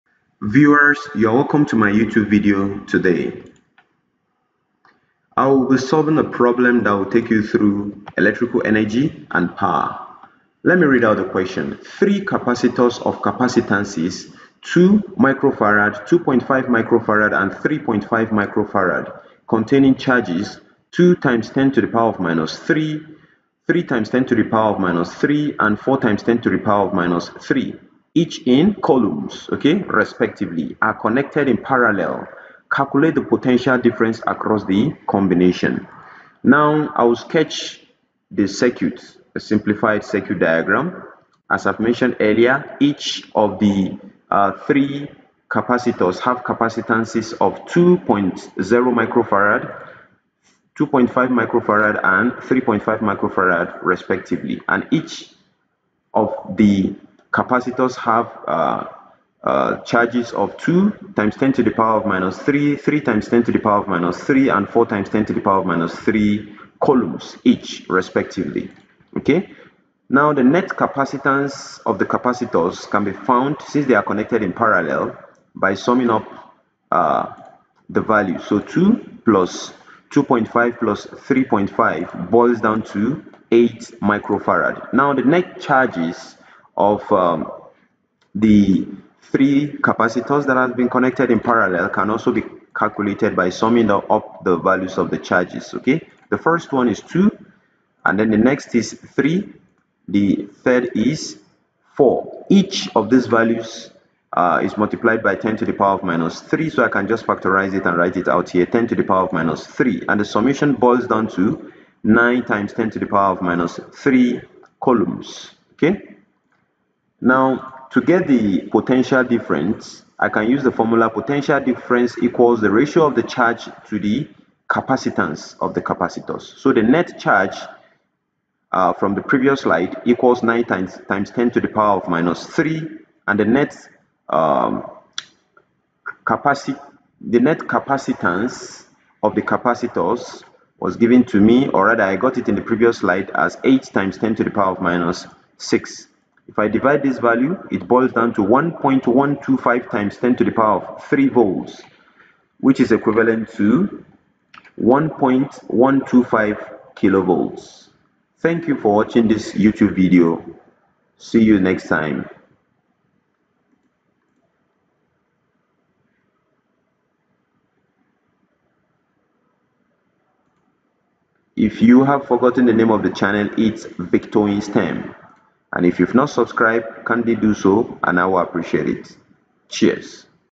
Electrical Energy and Power 2
Three capacitors of capacitance 2.0μF, 2.5μF, and 3.5 μF containing charges
2.0 x 10-3 C,
3.0 x 10-3 C, and
4.0 x 10-3 C, respectively, are connected in parallel. Calculate the potential difference across the combination.